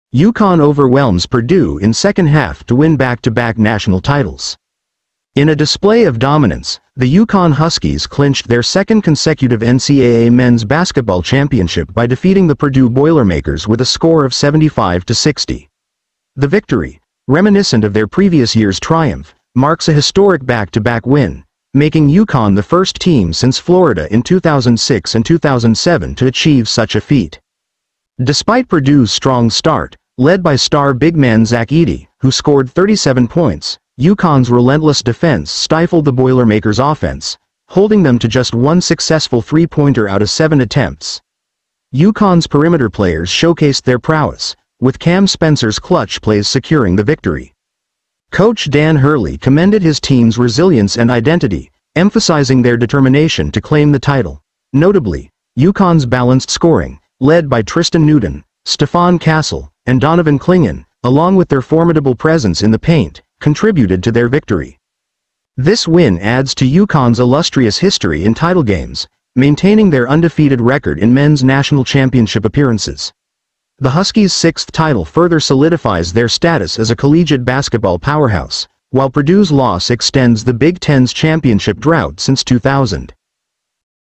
UConn overwhelms Purdue in second half to win back-to-back national titles.
In a display of dominance, the UConn Huskies clinched their second consecutive NCAA men's basketball championship by defeating the Purdue Boilermakers with a score of 75-60. The victory, reminiscent of their previous year's triumph, marks a historic back-to-back win, making UConn the first team since Florida in 2006 and 2007 to achieve such a feat.

Despite Purdue's strong start, led by star big man Zach Edey, who scored 37 points, UConn's relentless defense stifled the Boilermakers' offense, holding them to just one successful 3-pointer out of seven attempts. UConn's perimeter players showcased their prowess, with Cam Spencer's clutch plays securing the victory.

Coach Dan Hurley commended his team's resilience and identity, emphasizing their determination to claim the title. Notably, UConn's balanced scoring, led by Tristen Newton, Stephon Castle, and Donovan Clingan, along with their formidable presence in the paint, contributed to their victory.

This win adds to UConn's illustrious history in title games, maintaining their undefeated record in men's national championship appearances. The Huskies' sixth title further solidifies their status as a collegiate basketball powerhouse, while Purdue's loss extends the Big Ten's championship drought since 2000.